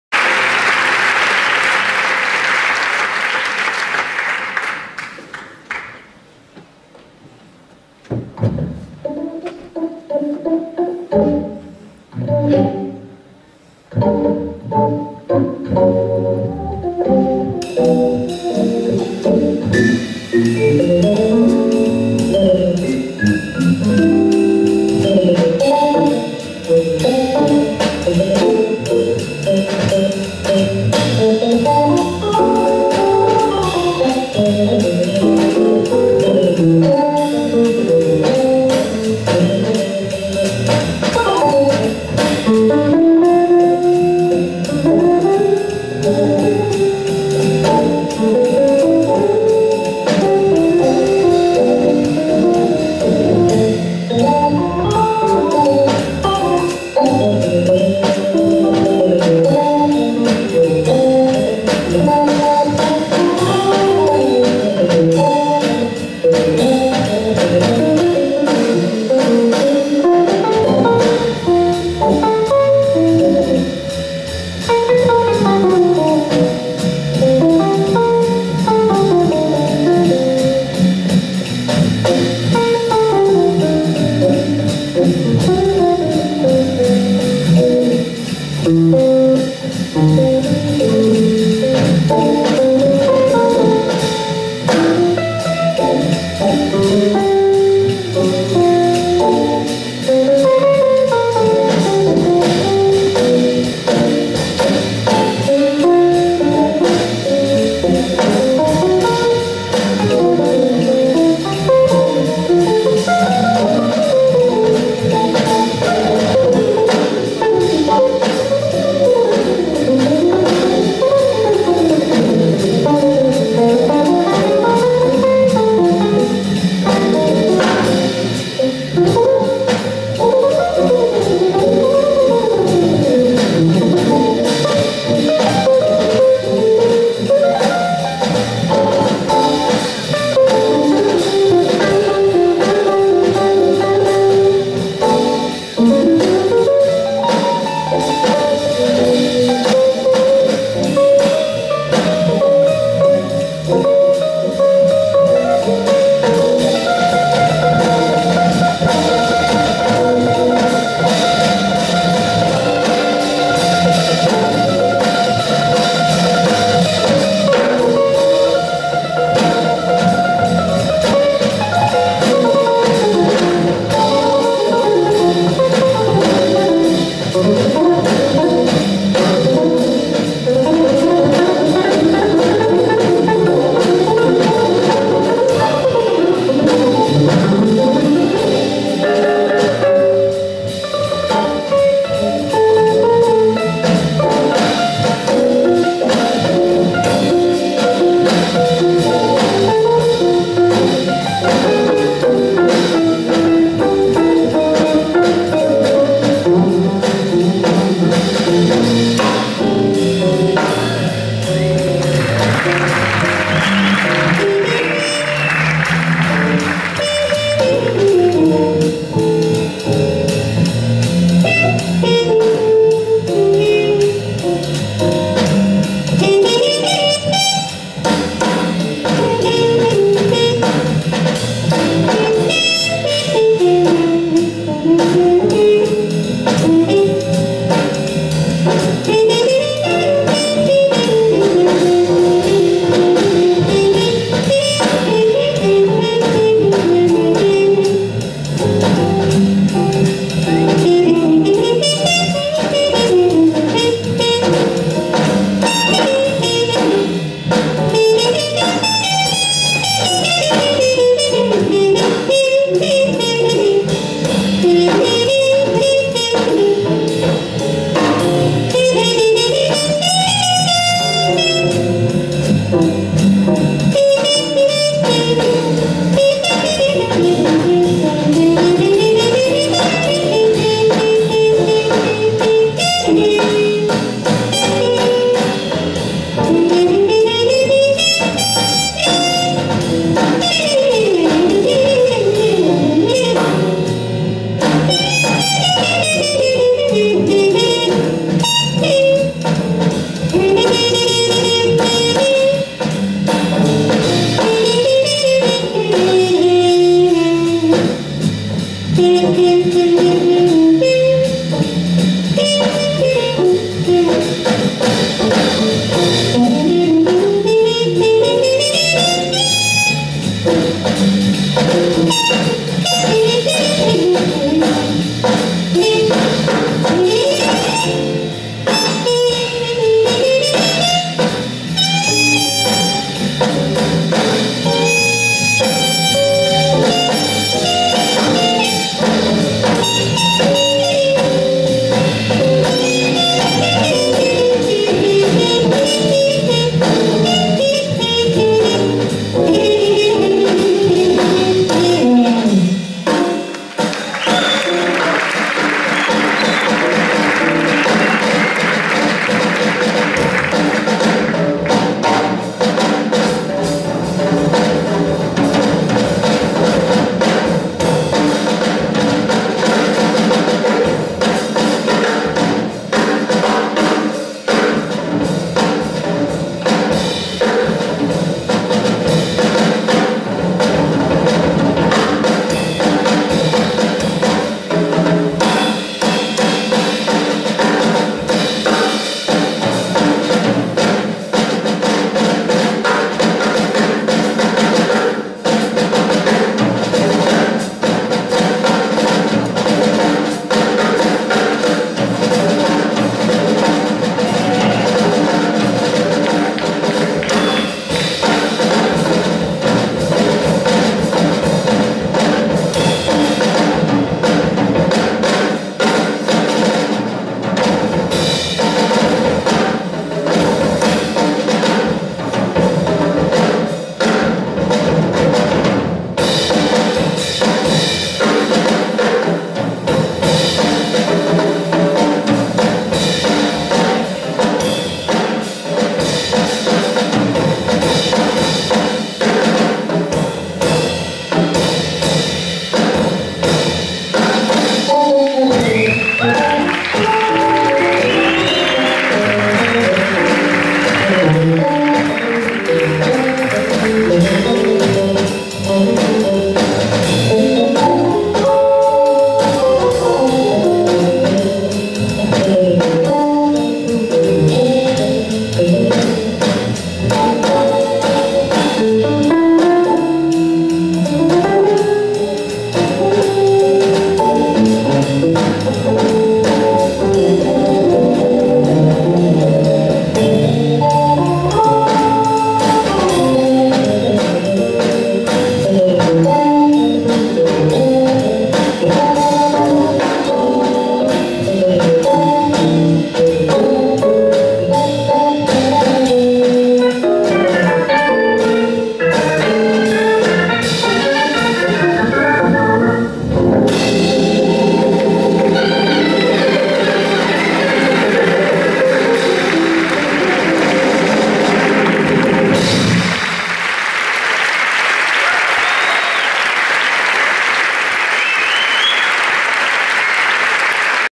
Jimmy Cobb Trio
Jimmy Cobb Trío en el Teatro Victoria Eugenia el 20 de julio de 2012 dentro de la programación del 47 Heineken Jazzaldia de San Sebastián

Jimmy Cobb (batería), Larry Coryell (guitarras), Joey De Francesco (órgano, trompeta)